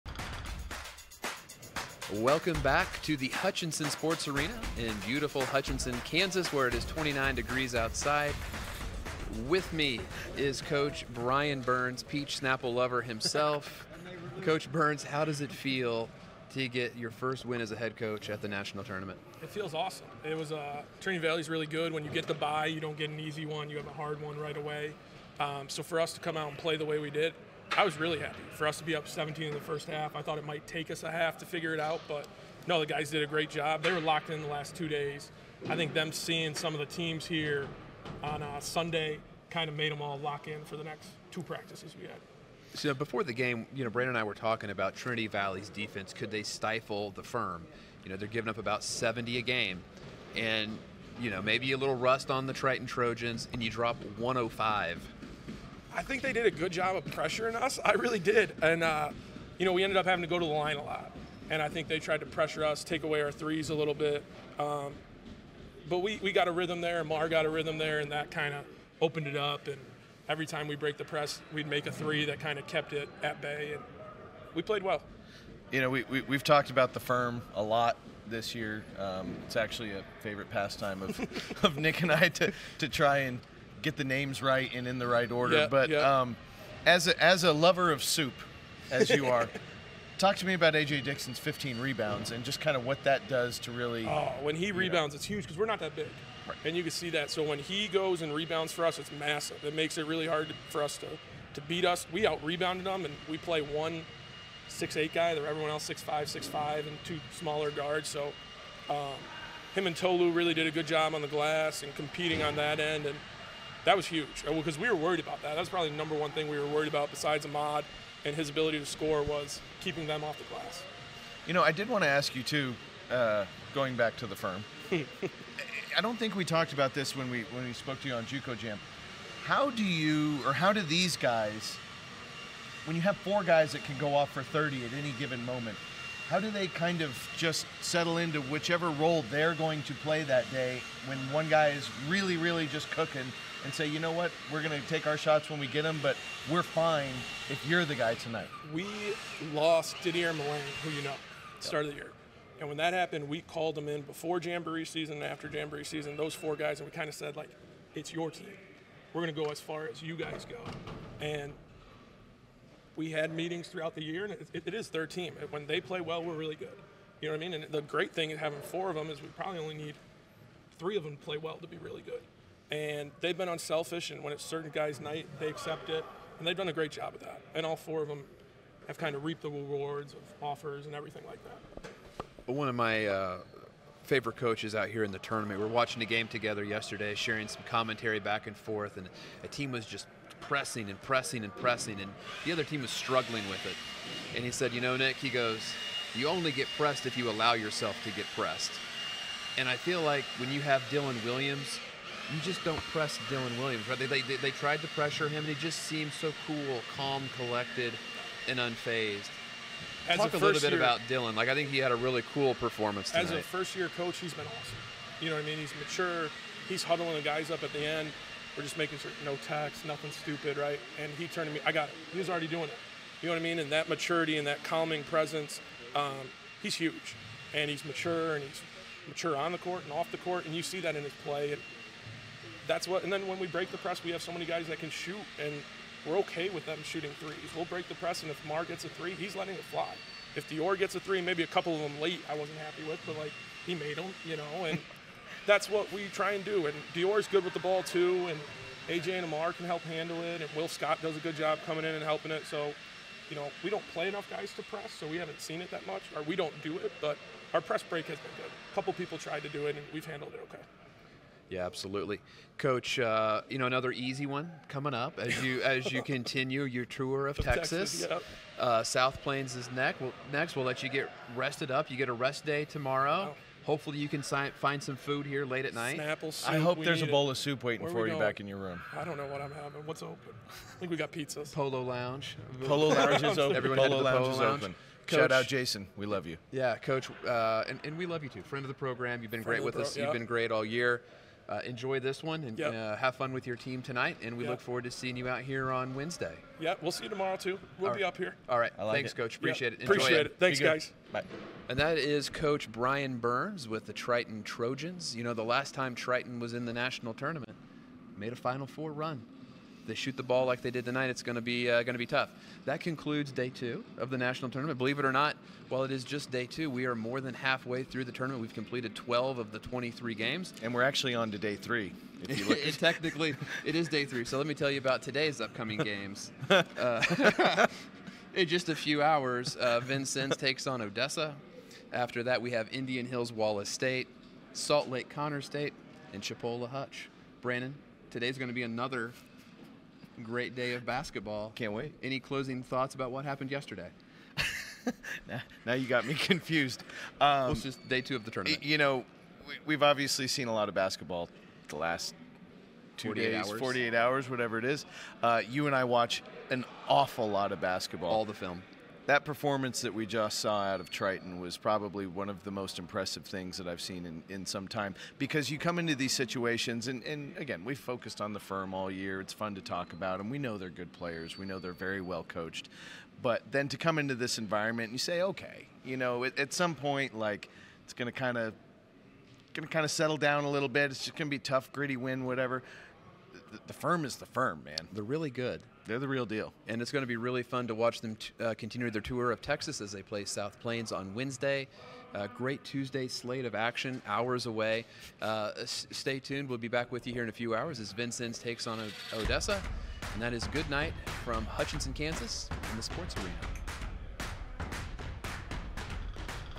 NJCAA DI Men's Basketball Championship Studio Show - Day 2, Game 6: Triton vs. Trinity Valley
Brandon and Nic wrap up Day 2 in Hutchinson, Kansas and look forward to the final 4 games of the Sweet 16 on Day 3!